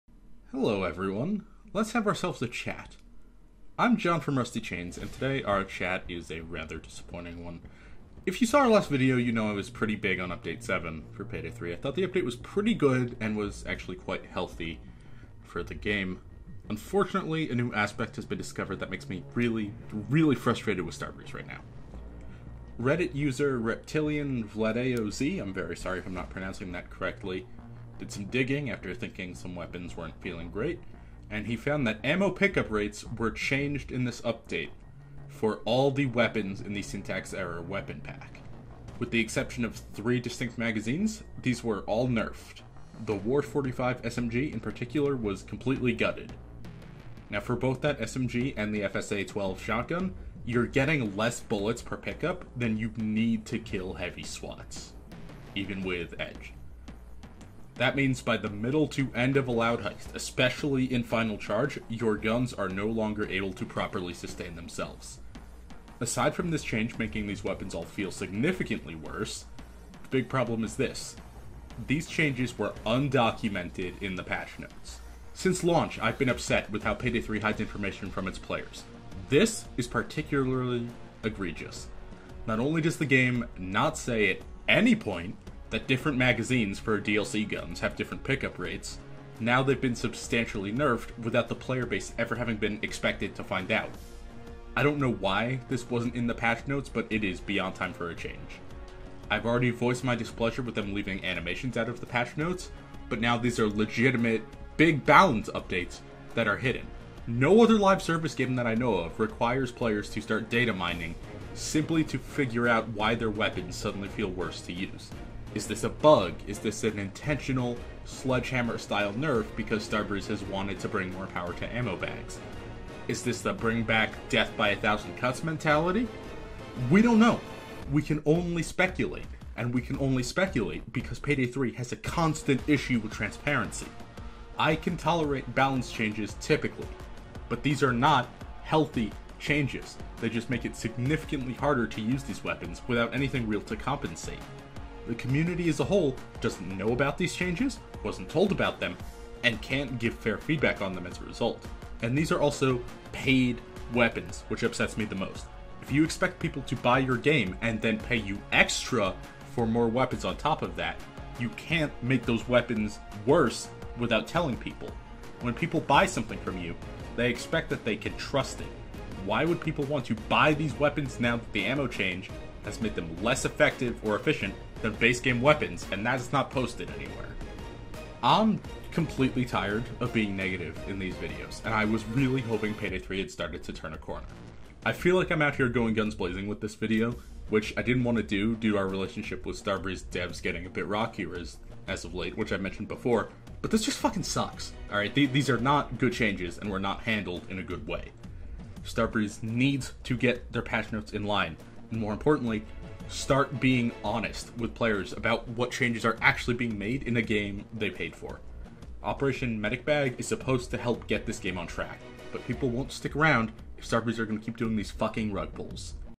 Starbreeze Nerfed Our Payday 3 Weapons Without Telling Us
DLC weapons for Payday with majorly nerfed in the last update, and nobody knew. We're pretty unhappy.